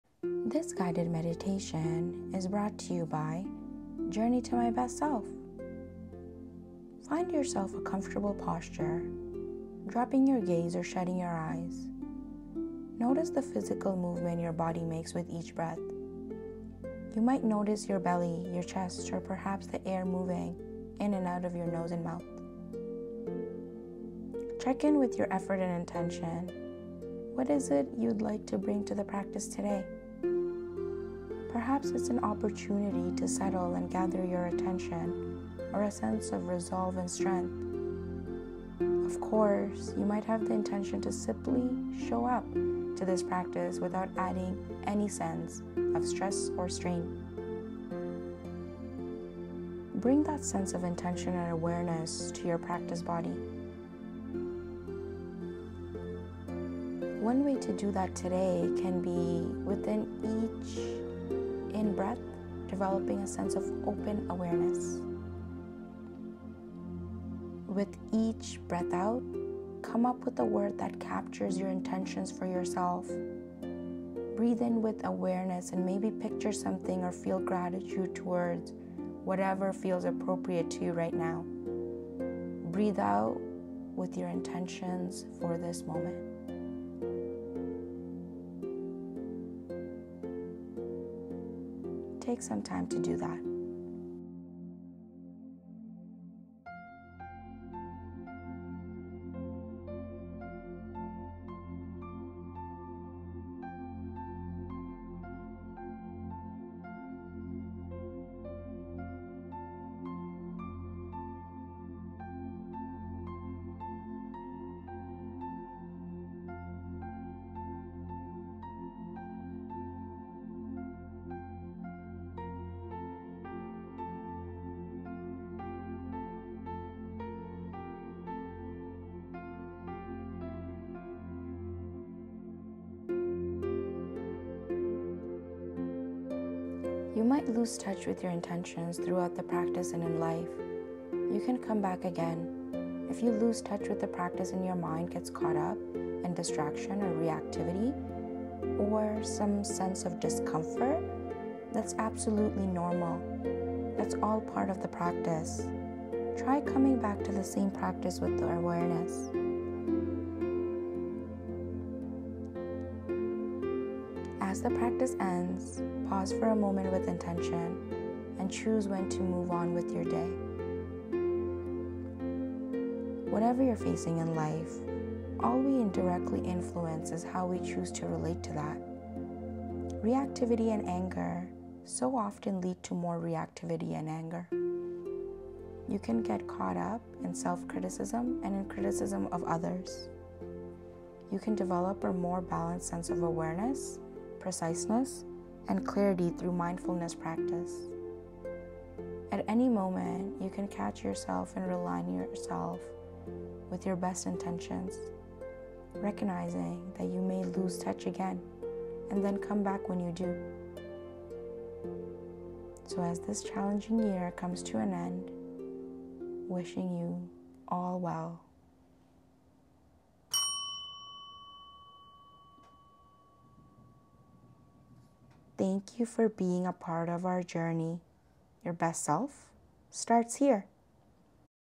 🧘 Setting Intentions for 2025 | New Year Guided Meditation
This new year guided meditation will help with setting intentions for 2025.

This intention meditation is ideal for stepping into the new year with the energy you want to experience in 2025.

Practicing this meditation regularly will help you take stock of your progress and achieve your goals.